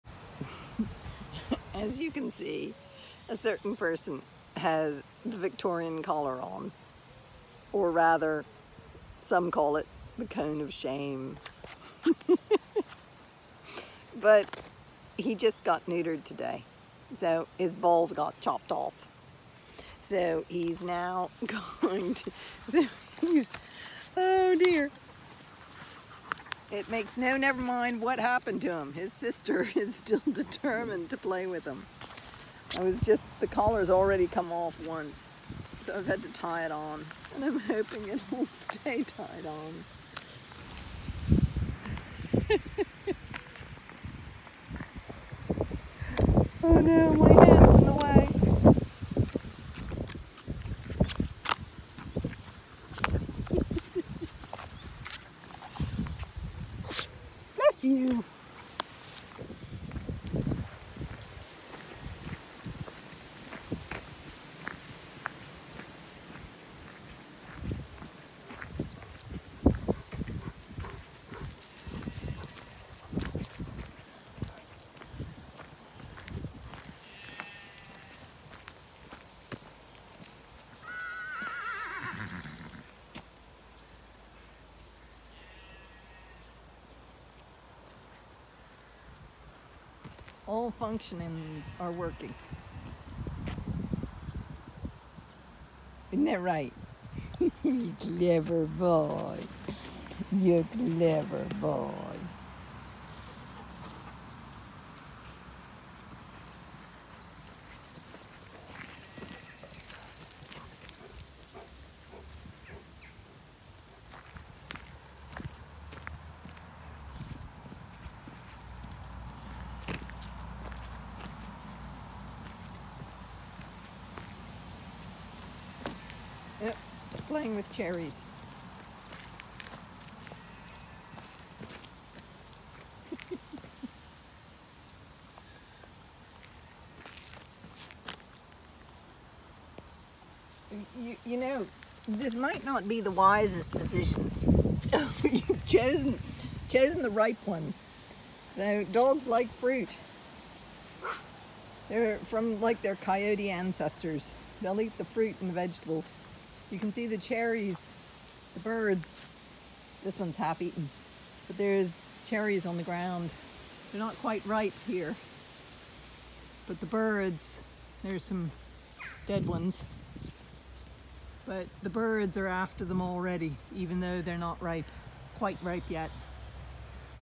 Spats lost his reproductive capacity so temporarily wears an Elizabethan cone
Always check that every exit orifice is functioning correctly after getting the snip.
All is well & cherries are ripening